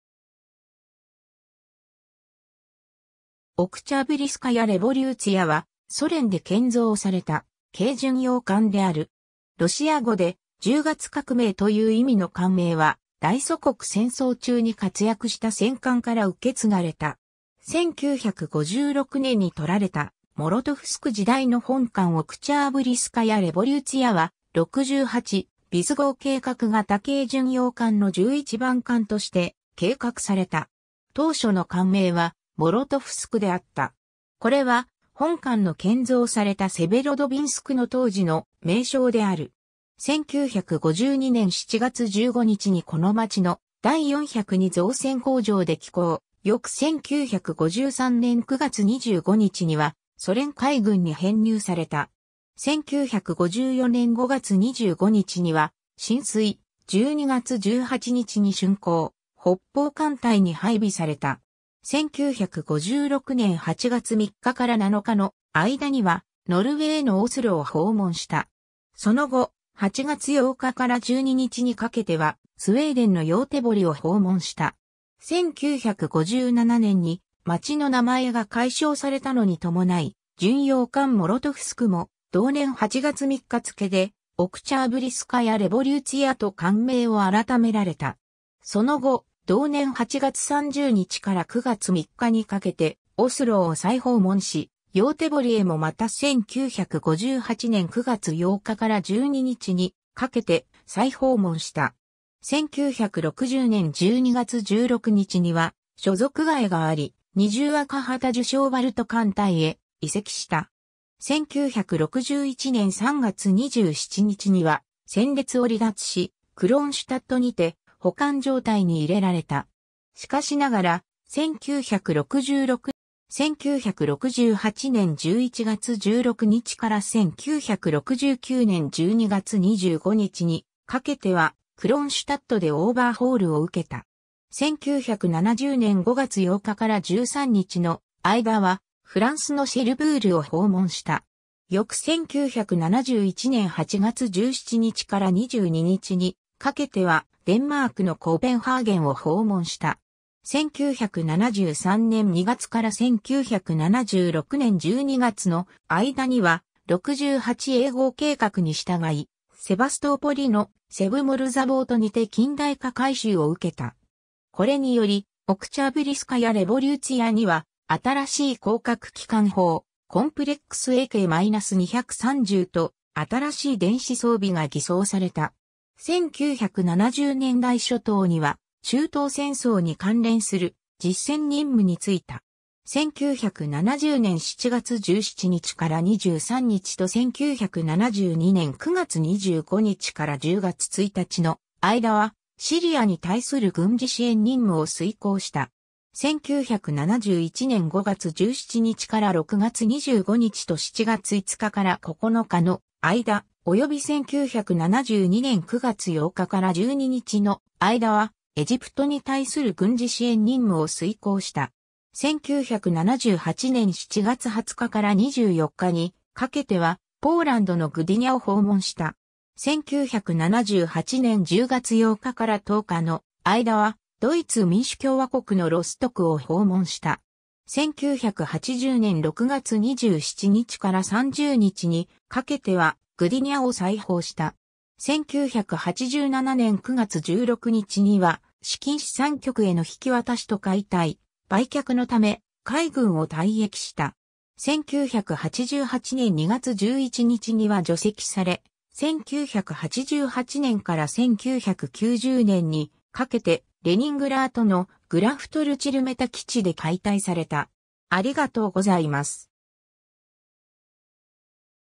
オクチャーブリスカヤ・レヴォリューツィヤ (軽巡洋艦)
オクチャーブリスカヤ・レヴォリューツィヤ(ロシア語:Октябрьская Революцияアクチャーブリスカヤ・リヴァリューツィヤ)は、ソ連で建造された軽巡洋艦(Лёгкий крейсер)である。
ロシア語で「十月革命」という意味の艦名は、大祖国戦争中に活躍した戦艦から受け継がれた。
 1956年に撮られた「モロトフスク」時代の本艦 オクチャーブリスカヤ・レヴォリューツィヤは、68-bis号計画型軽巡洋艦の11番艦として計画された。
当初の艦名は、モロトフスク(ロシア語:Молотовскモーラタフスク)であった。
これは、本艦の建造されたセヴェロドヴィンスクの当時の名称である。
1952年7月15日にこの町の第402造船工場で起工、翌1953年9月25日にはソ連海軍に編入された。
1954年5月25日には進水、12月18日に竣工、北方艦隊に配備された。
 1956年8月3日から7日の間には、ノルウェーのオスロを訪問した。
その後、8月8日から12日にかけてはスウェーデンのヨーテボリを訪問した。
1957年に町の名前が改称されたのに伴い、巡洋艦モロトフスクも同年8月3日付けでオクチャーブリスカヤ・レヴォリューツィヤと艦名を改められた。
その後、同年8月30日から9月3日にかけてオスロを再訪問し、ヨーテボリへもまた1958年9月8日から12日にかけて再訪問した。
 1960年12月16日には所属変えがあり、二重赤旗受賞バルト艦隊へ移籍した。
1961年3月27日には戦列を離脱し、クロンシュタットにて保管状態に入れられた。
しかしながら、1966年4月29日には戦列へ復帰した。
1968年11月16日から1969年12月25日にかけては、クロンシュタットでオーバーホールを受けた。
 1970年5月8日から13日の間は、フランスのシェルブールを訪問した。
翌1971年8月17日から22日にかけては、デンマークのコペンハーゲンを訪問した。
 1973年2月から1976年12月の間には、68-A号計画に従いセヴァストーポリの「セヴモルザヴォート」にて近代化改修を受けた。
これにより、オクチャーブリスカヤ・レヴォリューツィヤには新しい高角機関砲コンプレックスAK-230と新しい電子装備が艤装された。
 1970年代初頭には、中東戦争に関連する実戦任務に就いた。
1970年7月17日から23日と1972年9月25日から10月1日の間は、シリアに対する軍事支援任務を遂行した。
1971年5月17日から6月25日と7月5日から9日の間、および1972年9月8日から12日の間は、エジプトに対する軍事支援任務を遂行した。
 1978年7月20日から24日にかけては、ポーランドのグディニャを訪問した。
1978年10月8日から10日の間は、ドイツ民主共和国のロストクを訪問した。
1980年6月27日から30日にかけては、グディニャを再訪した。
 1987年9月16日には、資金資産局への引渡しと解体・売却のため、海軍を退役した。
1988年2月11日には除籍され、1988年から1990年にかけてレニングラート(現サンクトペテルブルク)の「グラフトルチェルメタ」基地で解体された。